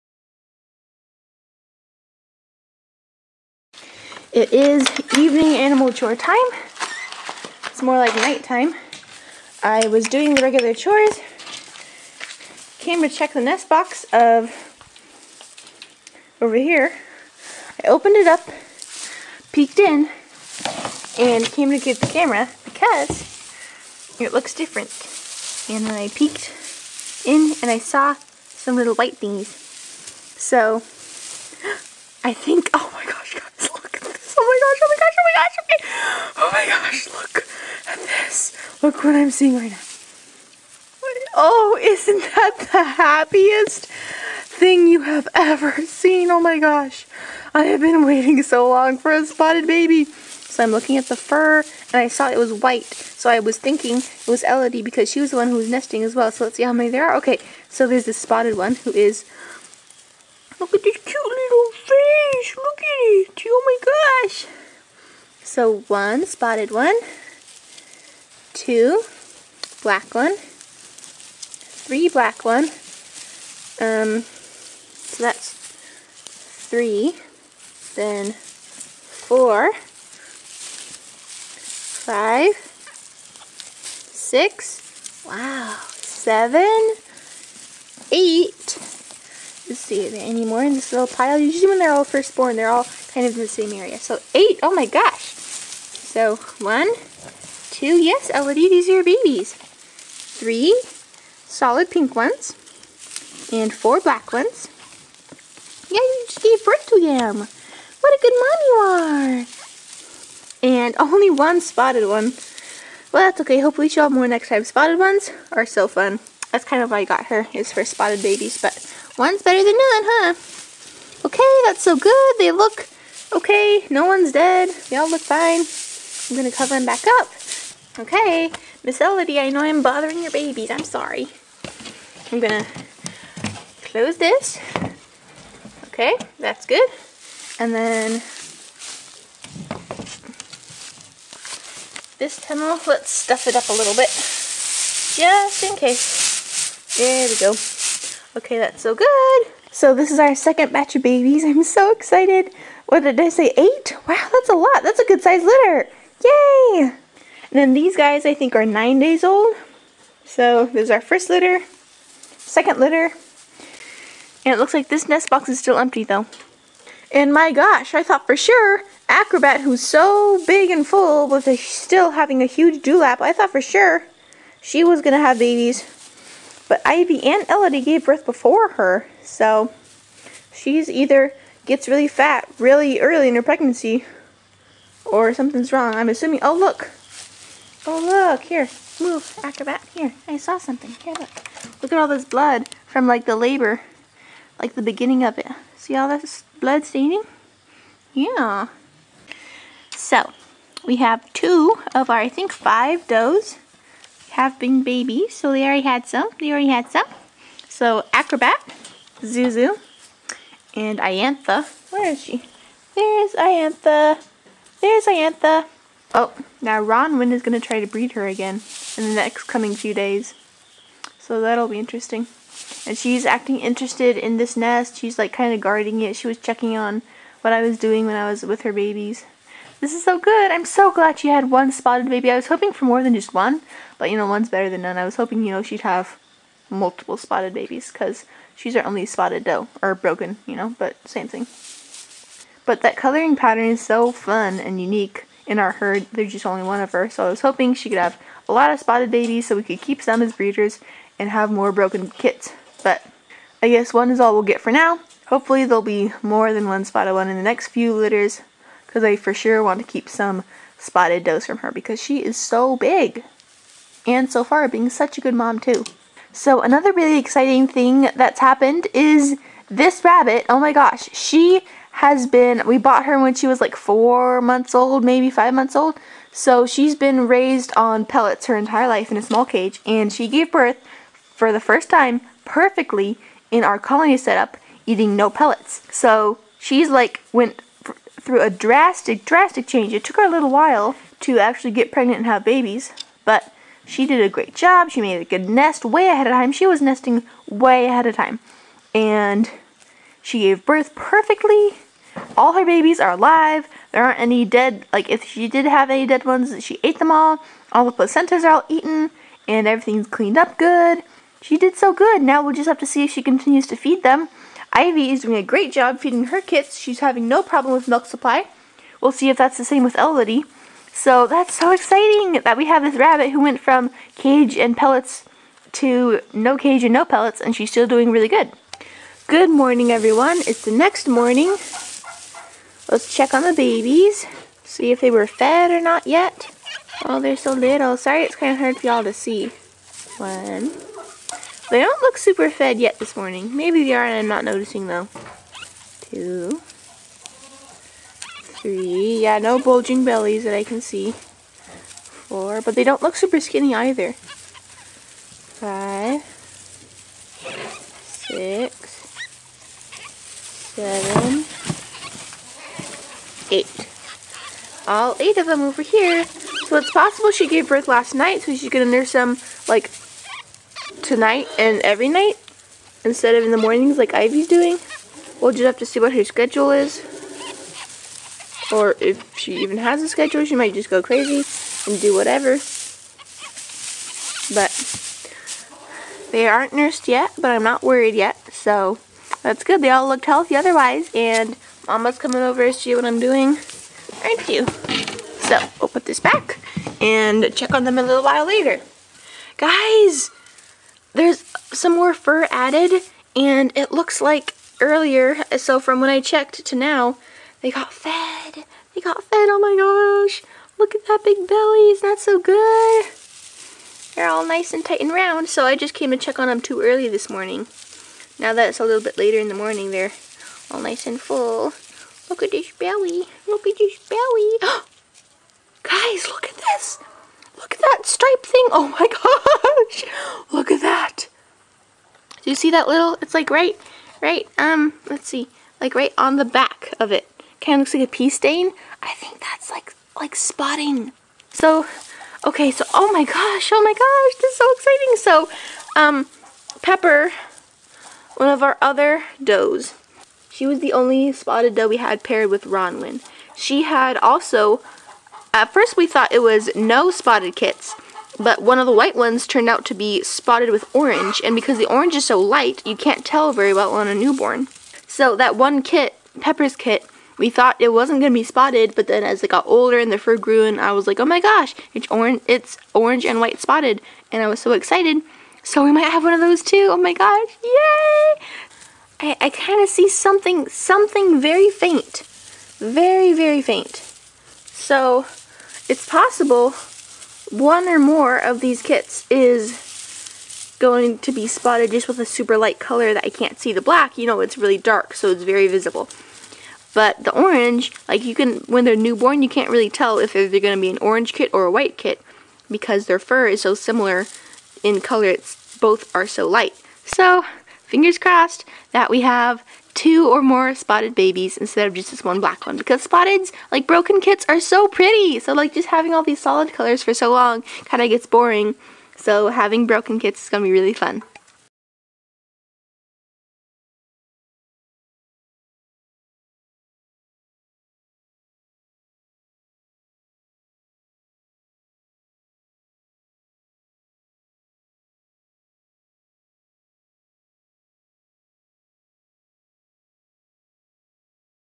Elody FINALLY Gave Birth! | Second Batch of Meat Rabbit Babies Born in the Colony This Fall | 2021
In this video I discover Elody (the herd queen in our meat rabbit colony) finally had babies! She built a great nest, pulled lots of fur, and cleaned her kits off well. Making sure she feeds her kits over the next day or two is all I have to do, and I TOTALLY didn't expect the surprise about this litters coloring near the end of the video.